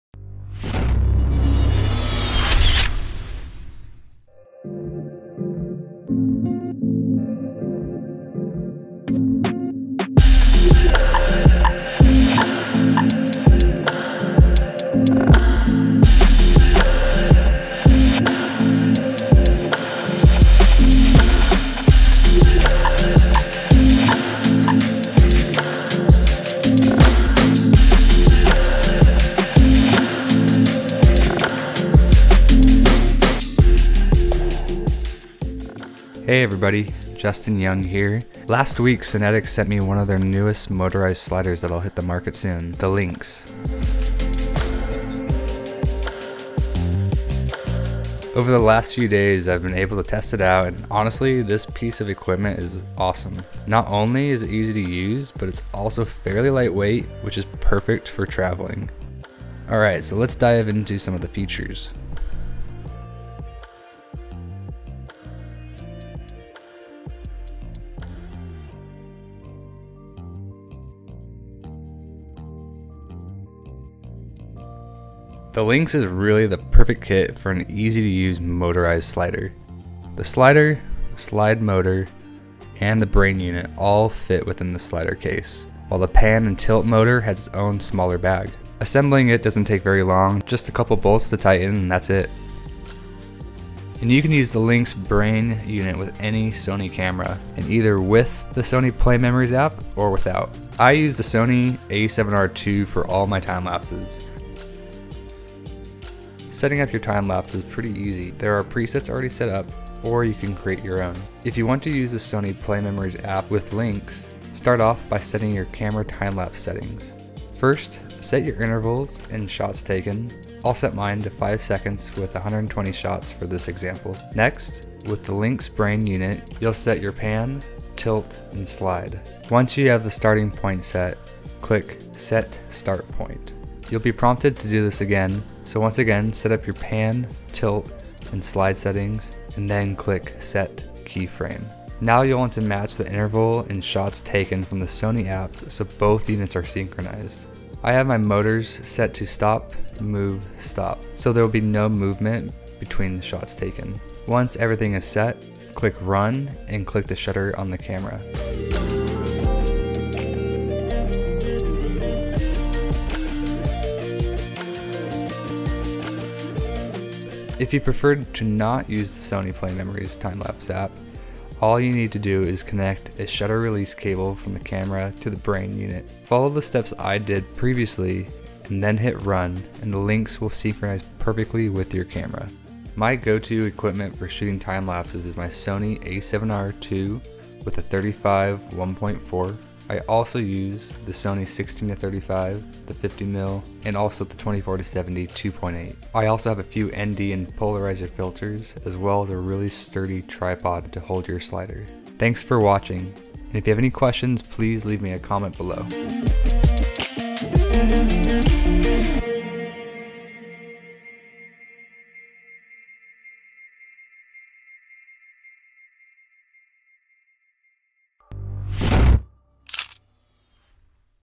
How to Shoot a Gorgeous Time-Lapse Using Cinetics Lynx Motorized Slider
How to Shoot a Gorgeous Time-Lapse Using Cinetics Lynx Motorized Slider. You'll be up and running soon with this quick tutorial of how to setup the Cinetics Lynx motorized slider for time-lapse, video, and stop-motion recordings.